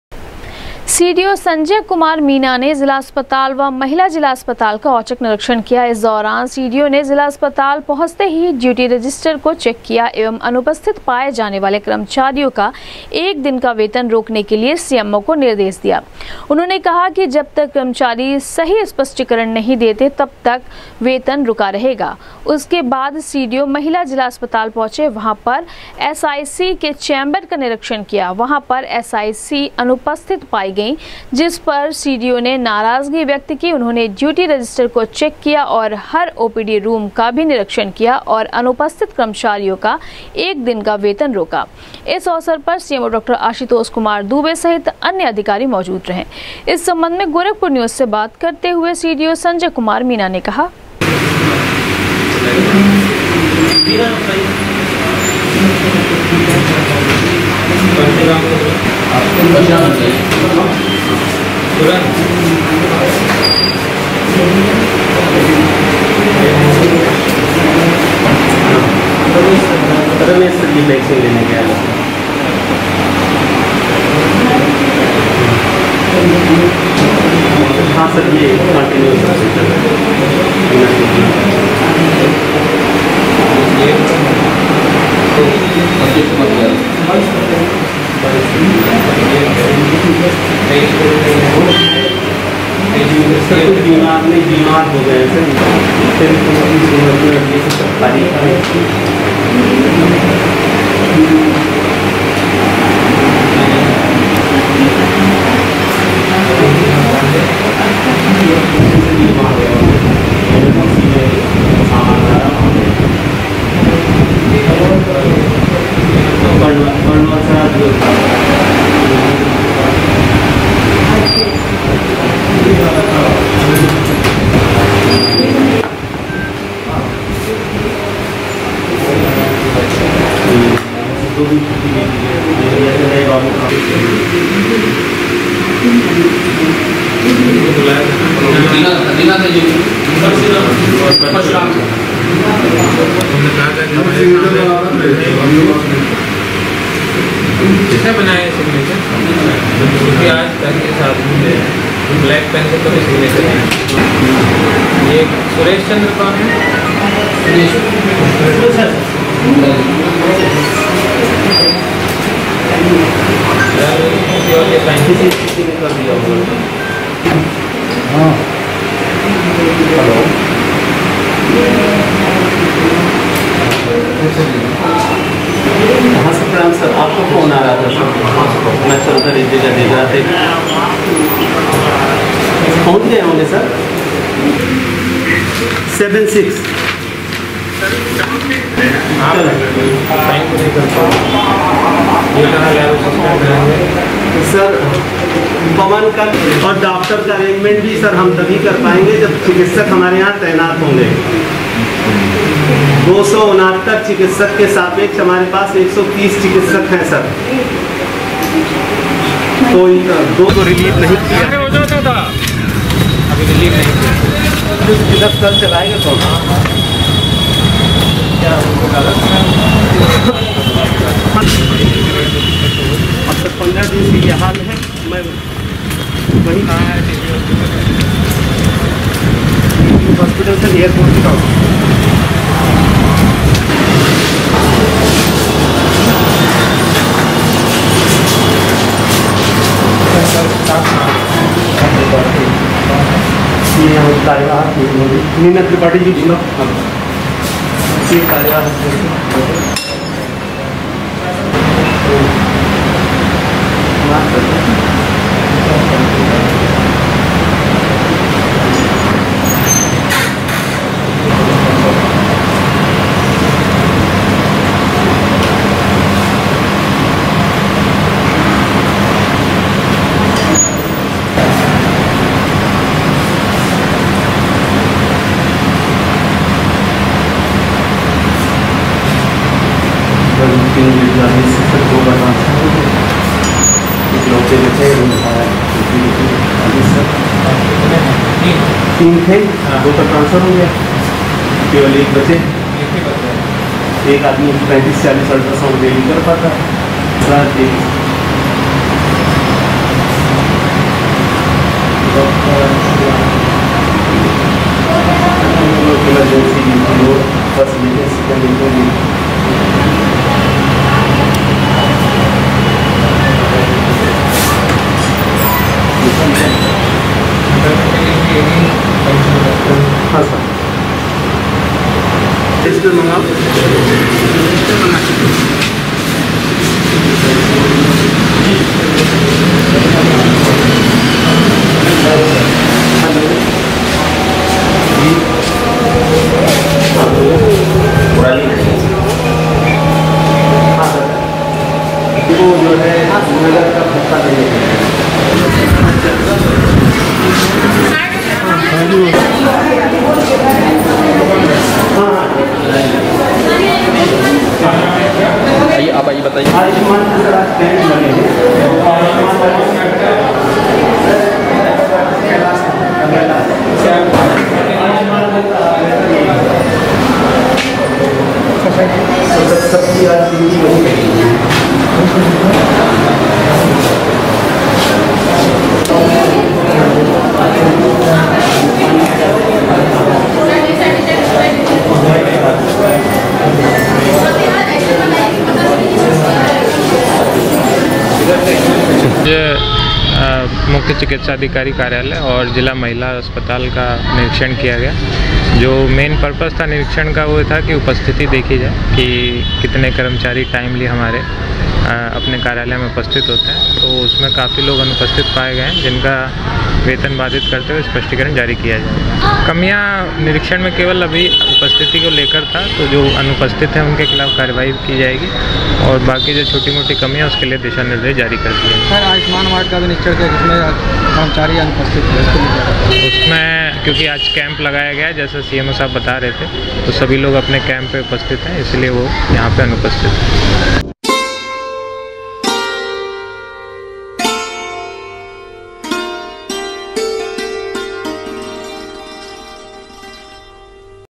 सीडीओ संजय कुमार मीना ने जिला अस्पताल व महिला जिला अस्पताल का किया औचक निरीक्षण,चेक किया ड्यूटी
सीडीओ संजय कुमार मीना ने जिला अस्पताल व महिला जिला अस्पताल का किया औचक निरीक्षण,चेक किया ड्यूटी रजिस्टर,अनुपस्थित पाये गये कर्मचारियों का एक दिन का वेतन किया बाधित